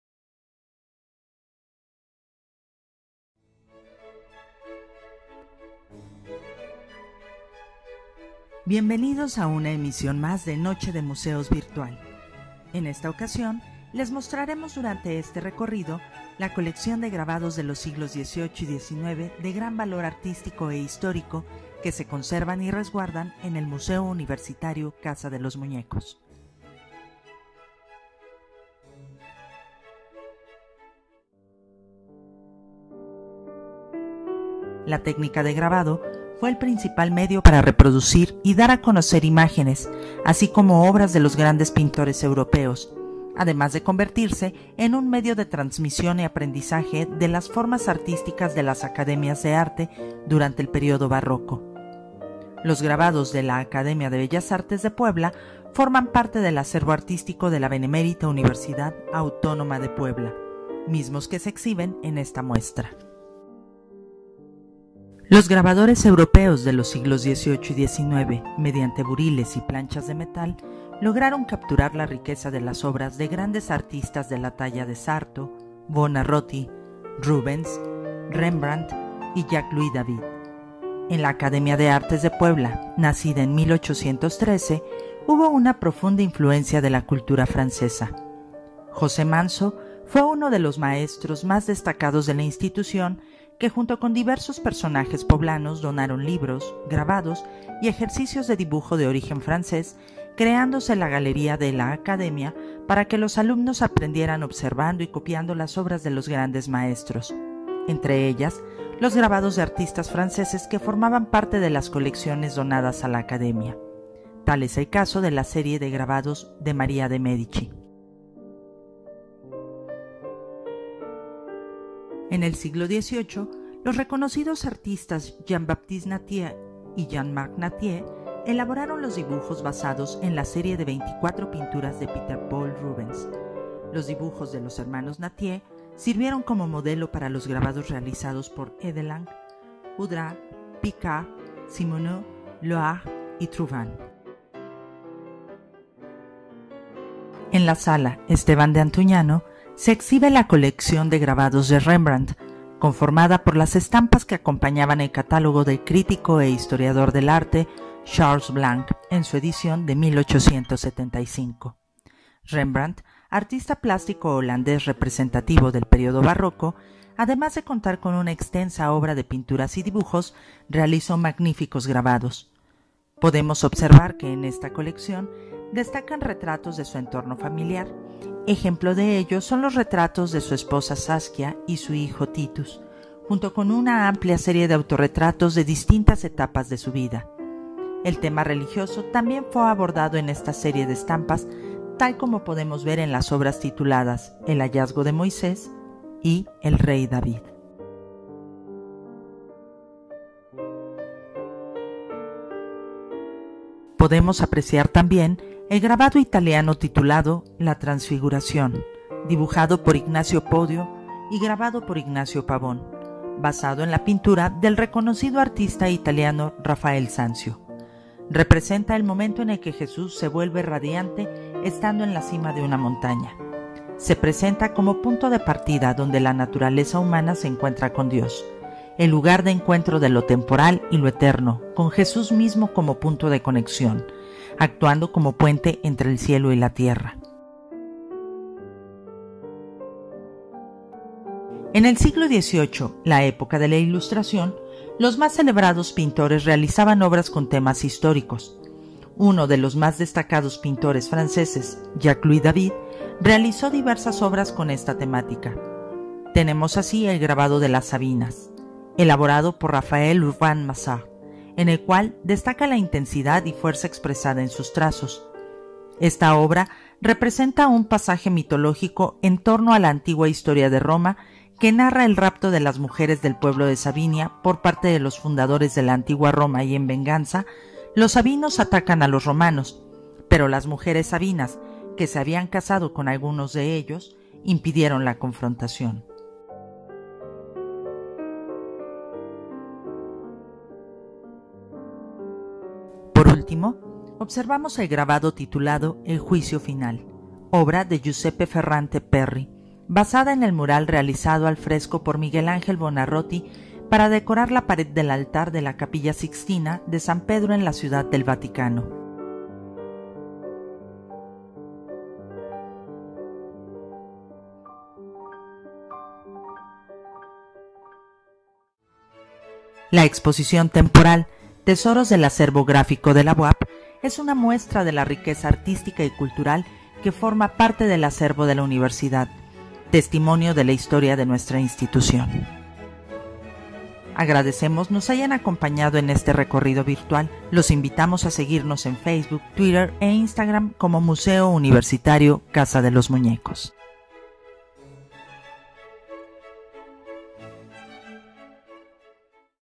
Exposición temporal: Tesoros del acervo gráfico de la BUAP
𝙏𝙚𝙨𝙤𝙧𝙤𝙨 𝙙𝙚𝙡 𝙖𝙘𝙚𝙧𝙫𝙤 𝙜𝙧𝙖́𝙛𝙞𝙘𝙤 𝙙𝙚 𝙡𝙖 𝘽𝙐𝘼𝙋,  es una colección de grabados de los siglos XVIII y XIX de gran valor artístico e histórico que forman
parte del acervo del Museo Universitario Casa de los Muñecos.